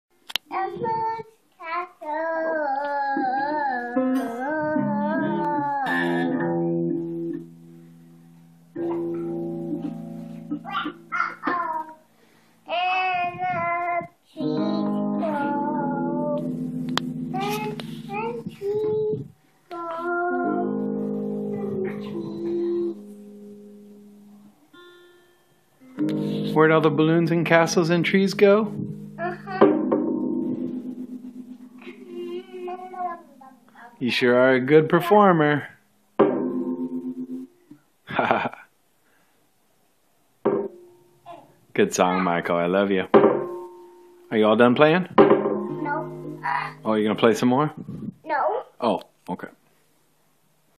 Where did all the balloons and castles go?
Michael's song about balloons and castles. I caught him late and he was done by then, but he has been working on this song about balloons and castles and today he added in trees too. Haha